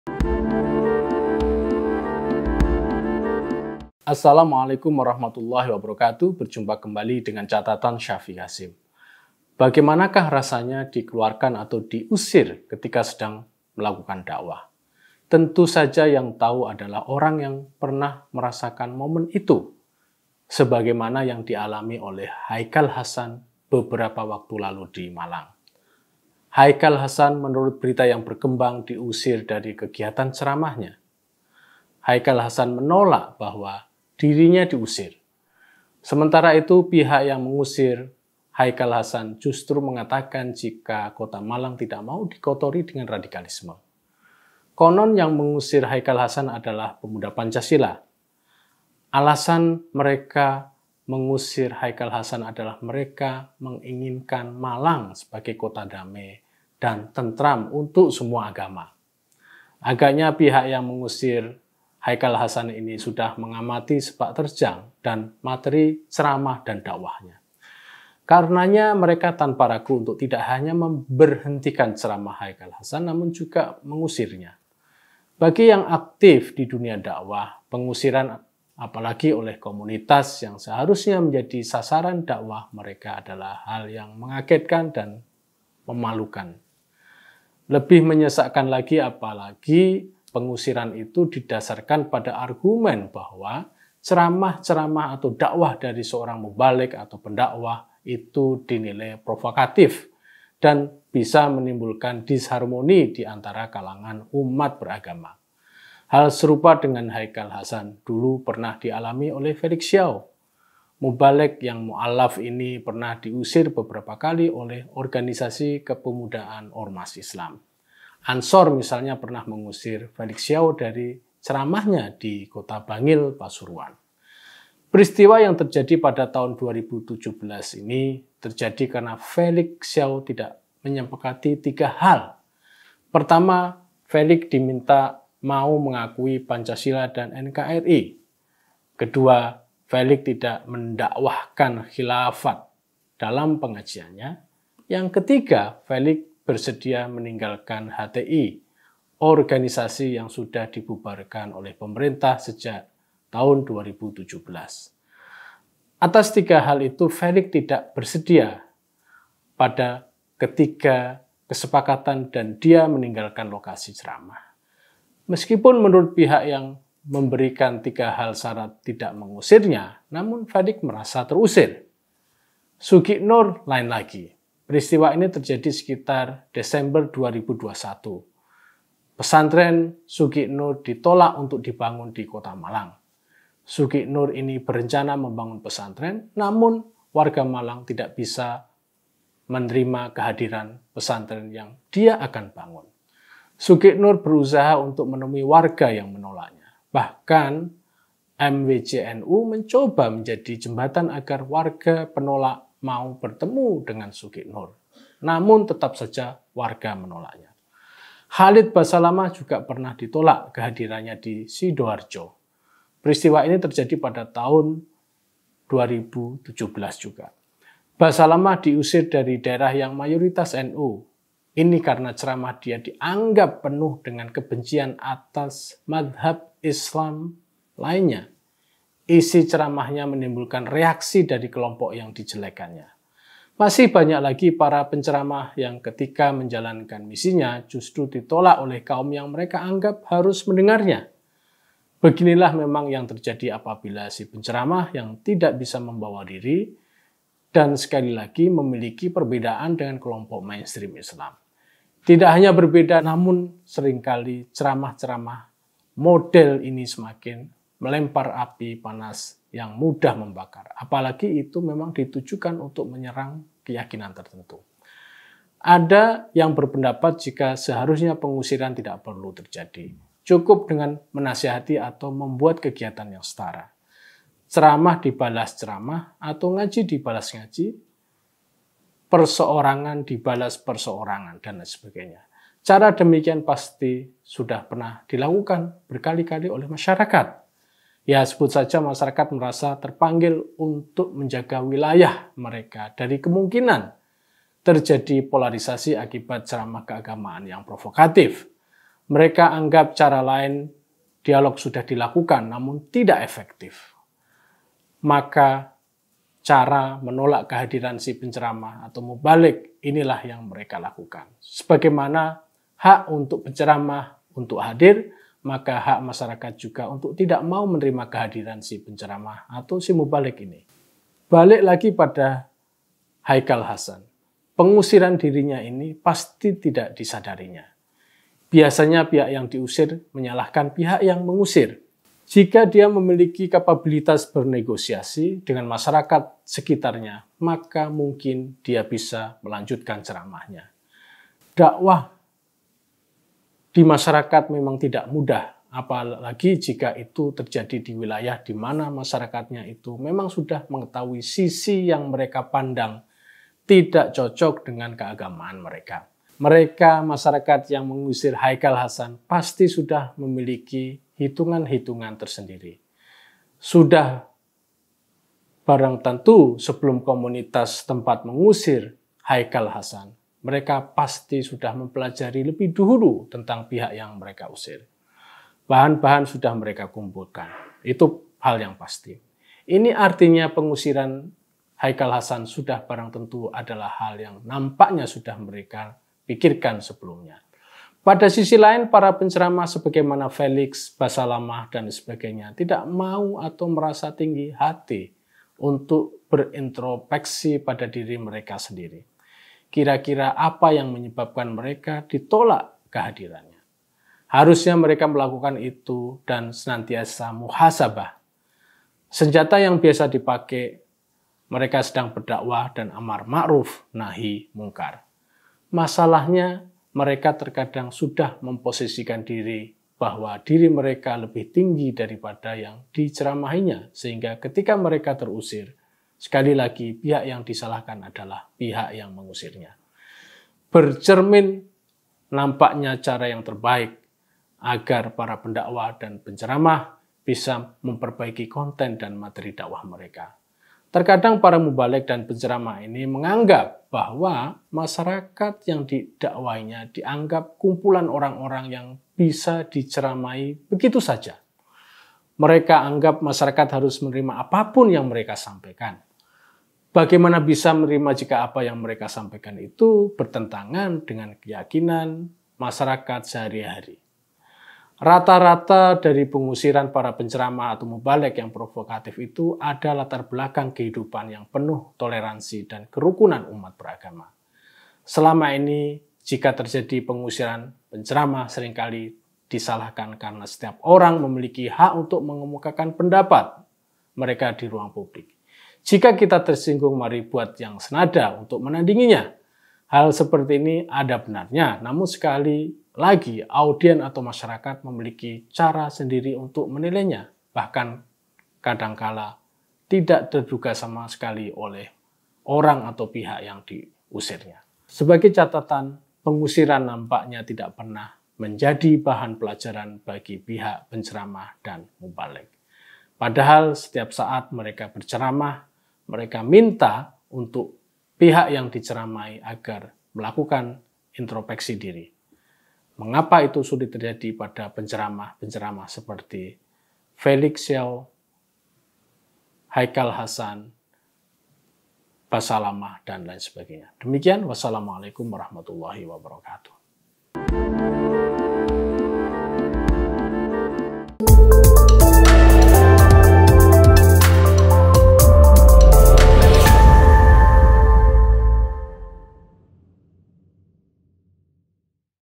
HAIKAL HASAN, FELIX, BASSALAMAH, USTADZ PROVOKATIF YANG DIUSIR WARGA I Catatan Syafiq Hasyim
Ulama yang bikin gaduh membuat jamaah harus mengusirnya. Contohnya Haikal Hasan, Felix Siauw, Khalid Bassalamah. Orang-orang seperti ini harusnya introspeksi diri.